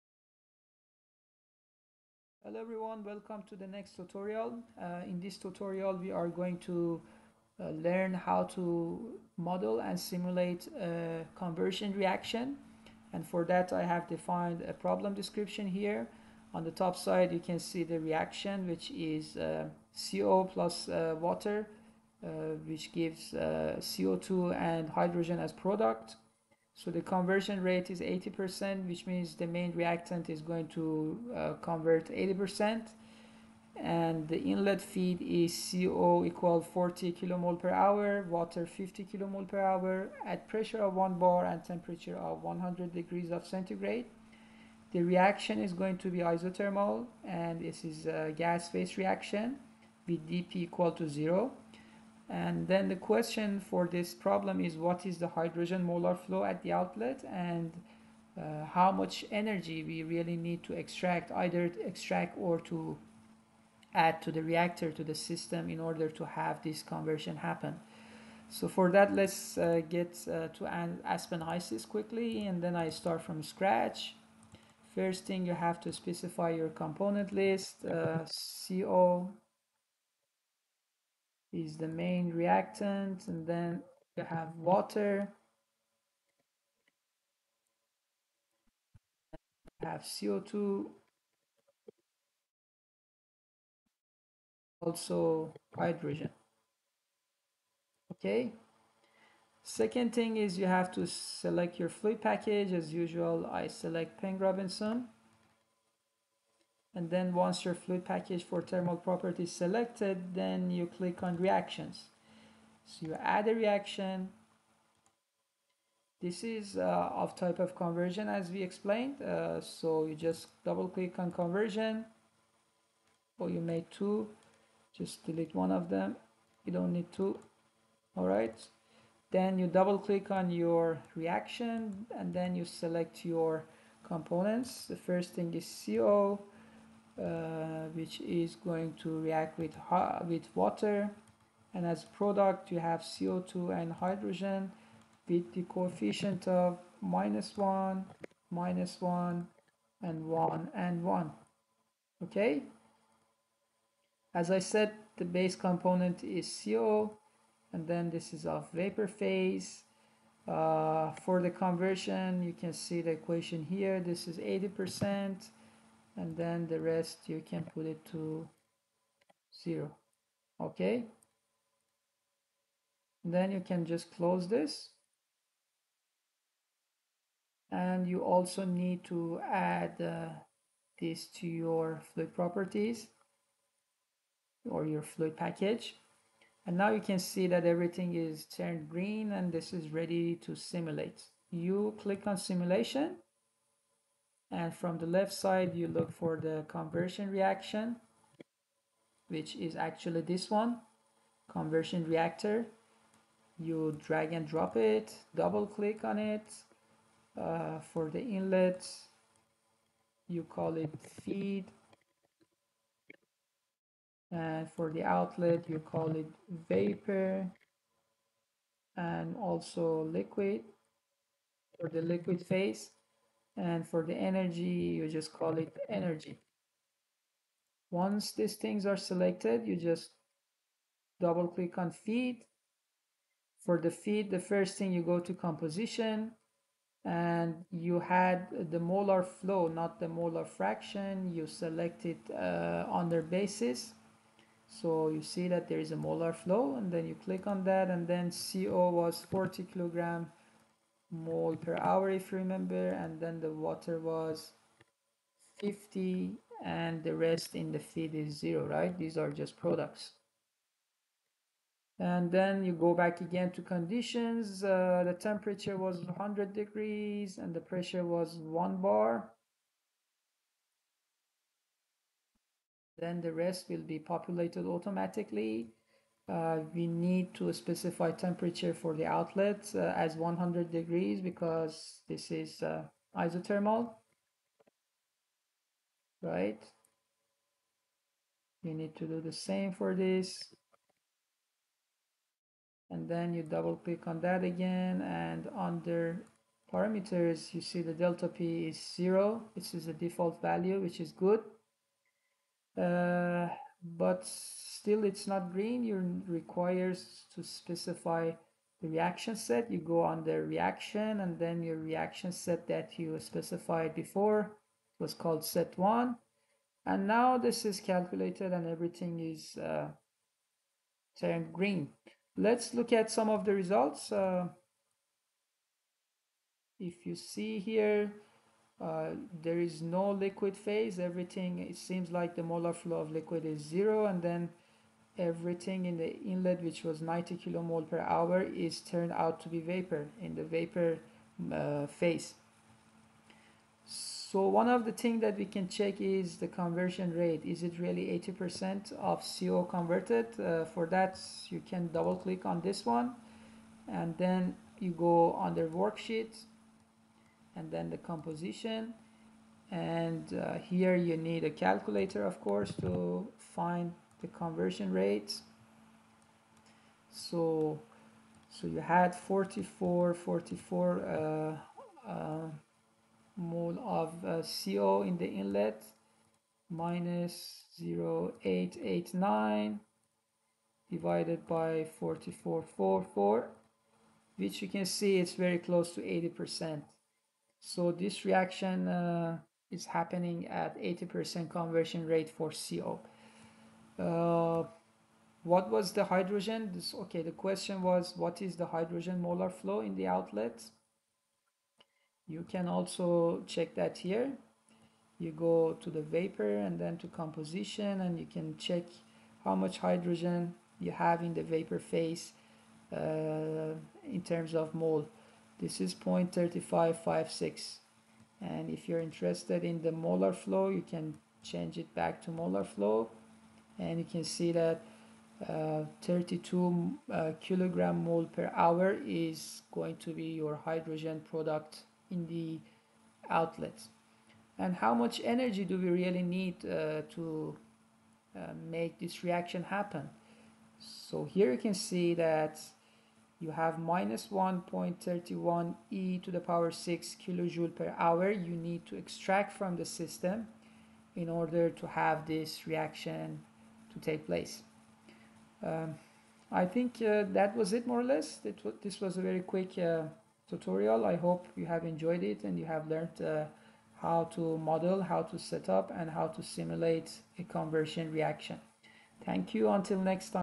Conversion Reaction in Aspen Hysys
How to set up and simulate Conversion Reaction in Aspen Hysys